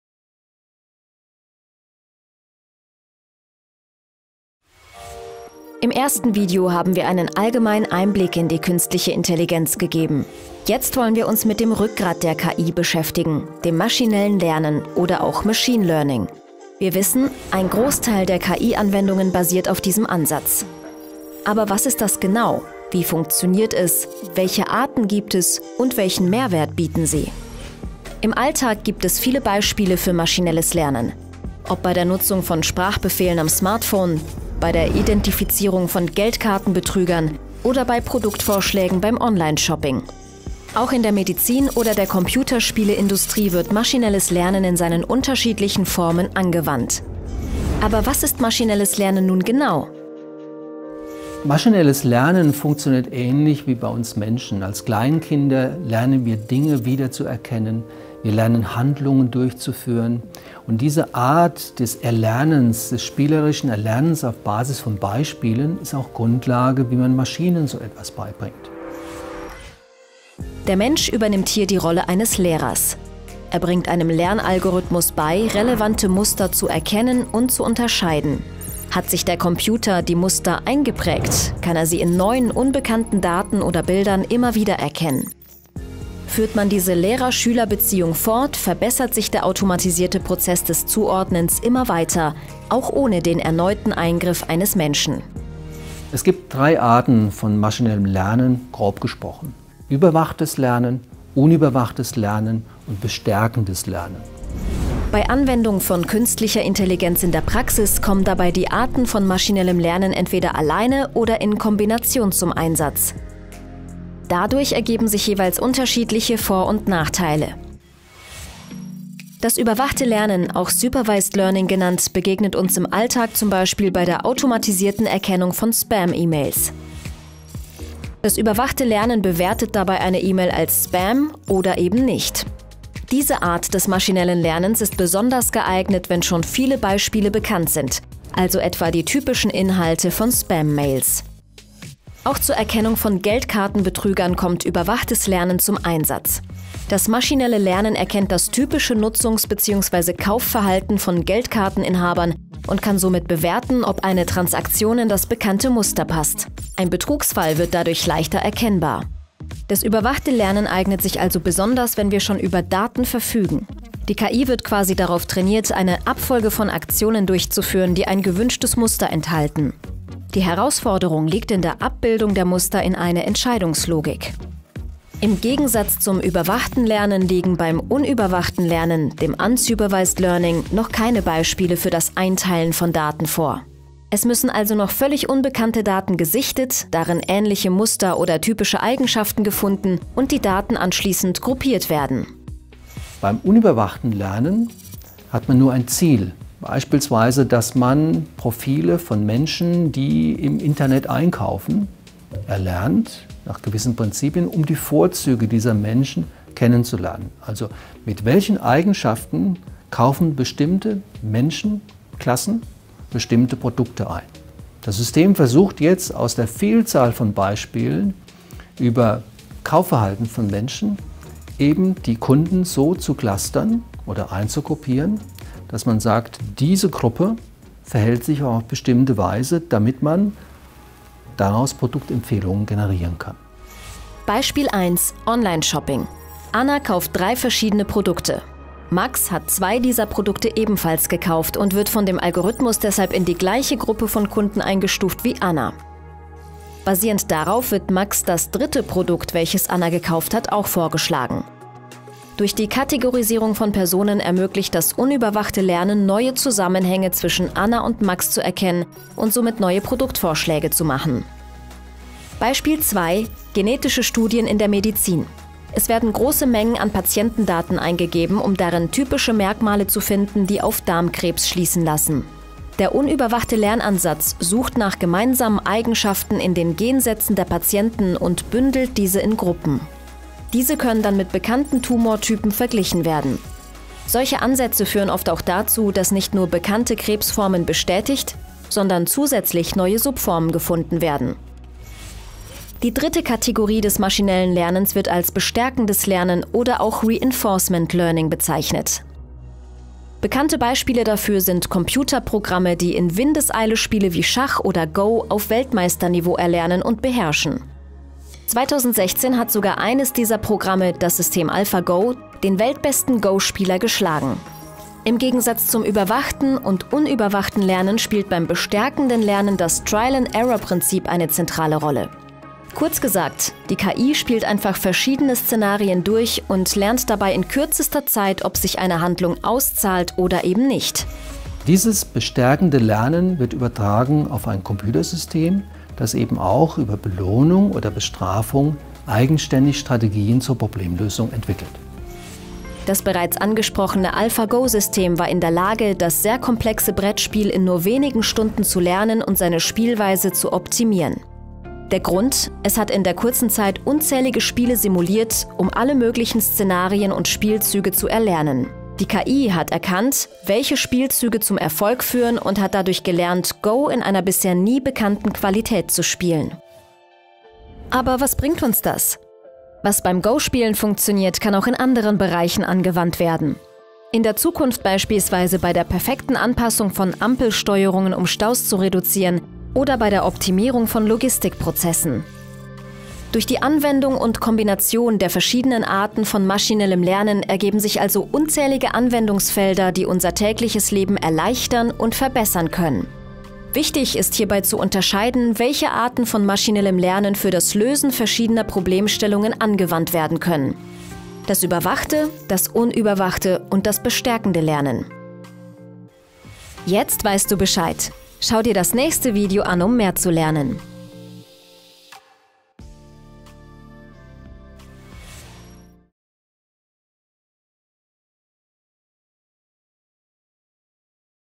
Künstliche Intelligenz - Was steckt dahinter?
Folge 2/4: Künstliche Intelligenz ist überall präsent. Mittlerweile nutzen wir sie täglich - teilweise ohne es zu bemerken. Beispielweise bei der Entsperrung des Smartphones per Gesichtserkennung, bei der Nutzung von Instagram, Facebook und anderen Social Media Plattformen oder beim Online Shopping.

Aber was ist eigentlich Künstliche Intelligenz? Woher kommt der Begriff und was steckt dahinter? Und nicht zuletzt, welche Chancen und Risiken ergeben sich durch die Künstliche Intelligenz für jeden einzelnen von uns.

Dazu haben wir einen der bekanntesten deutschen Forscher auf dem Gebiet der Künstlichen Intelligenz befragt. Dr. Prof. h.c. Andreas Dengel forscht und lehrt seit 1993 auf dem Gebiet der Künstlichen Intelligenz und leitet das Deutsche Forschungszentrum für Künstliche Intelligenz in Kaiserslautern.